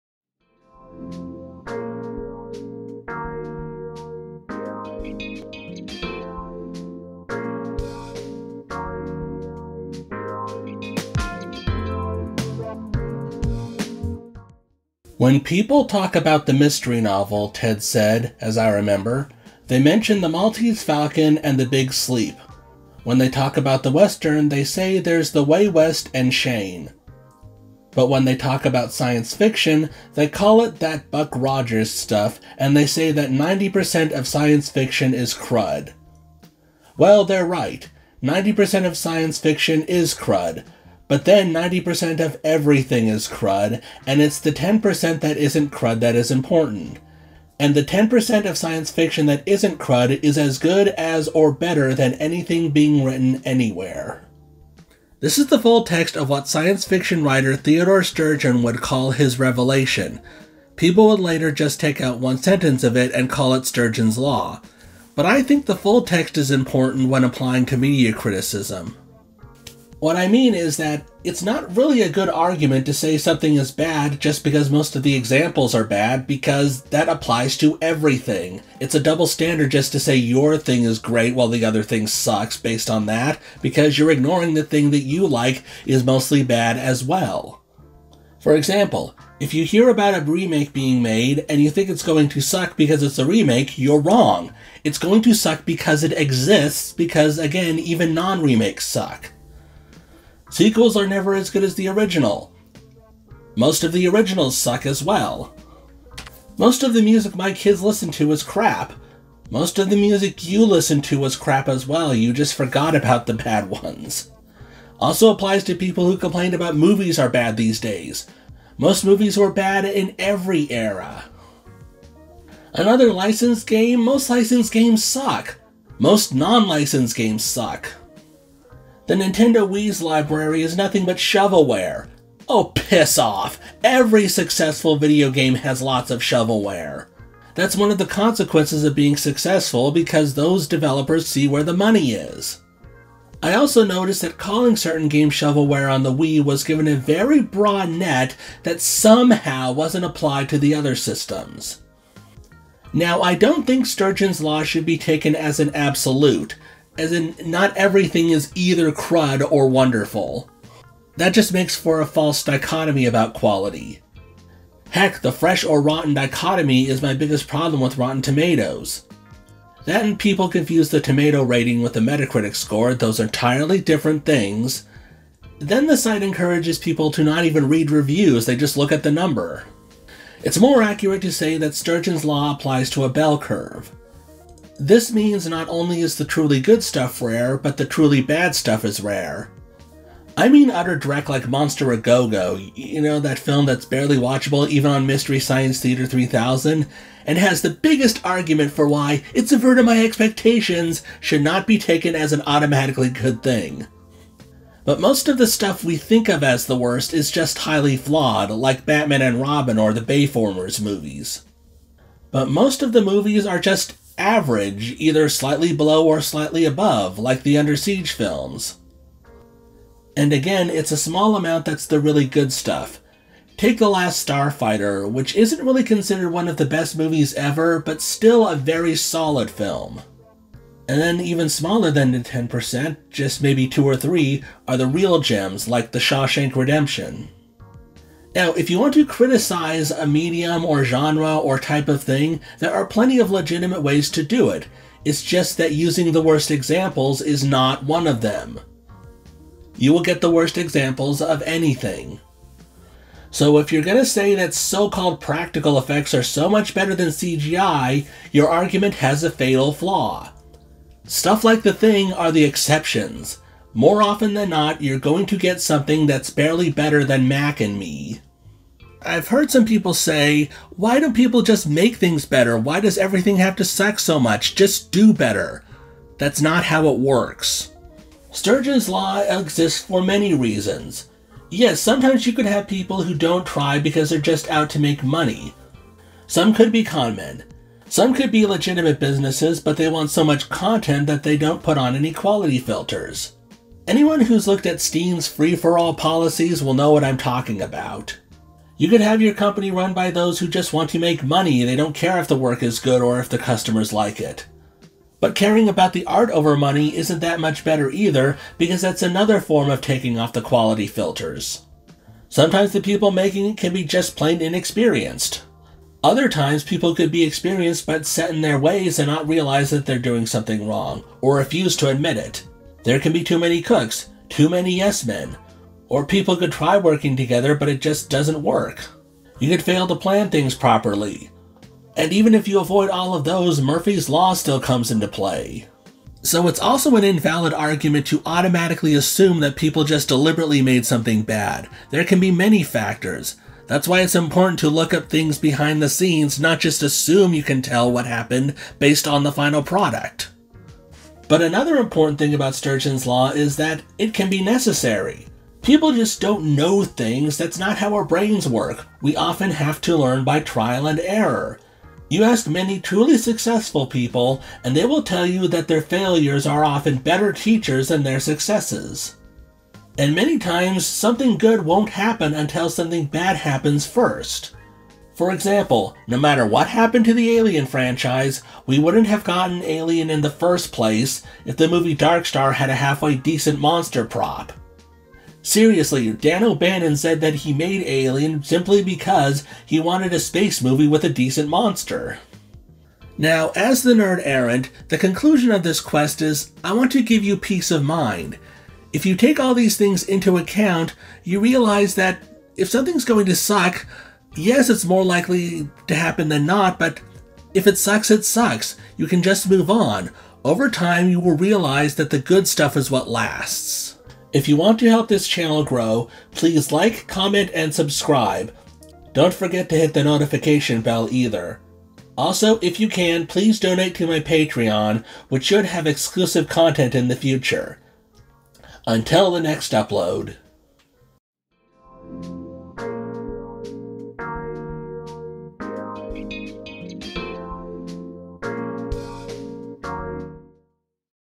Ignorance Of Sturgeon's Law Is No Excuse
Sturgeon's Law/Revelation tells us that if you bash something for being mostly crud, you have to remember that most of everything is crud. This unfortunately is not used often enough.